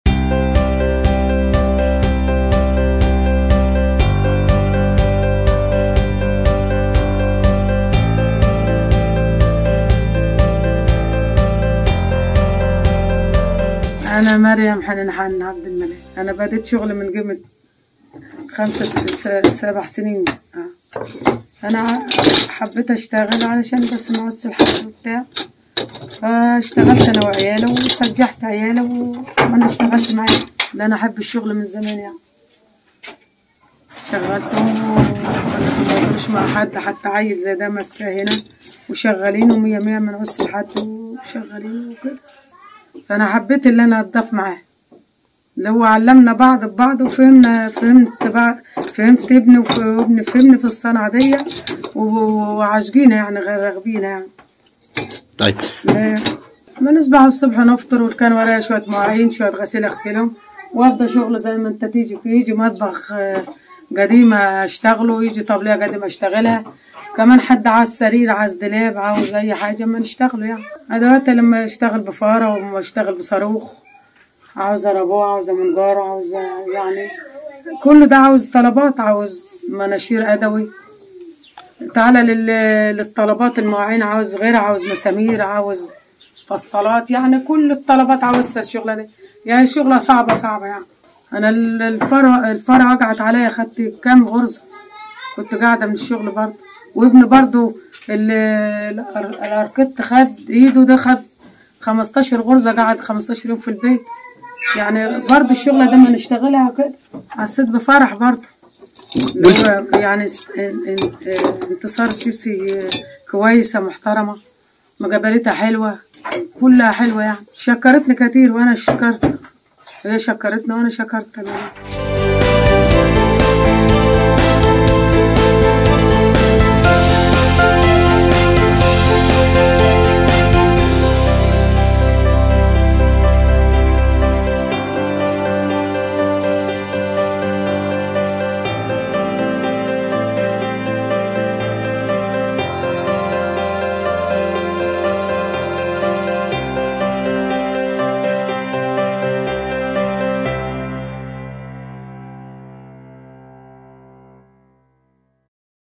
«نجارة المنيا» بعد تكريمها: بشقى وأتعب طول اليوم.. بس فرحانة ومستورة
تقرير : إسلام فهمي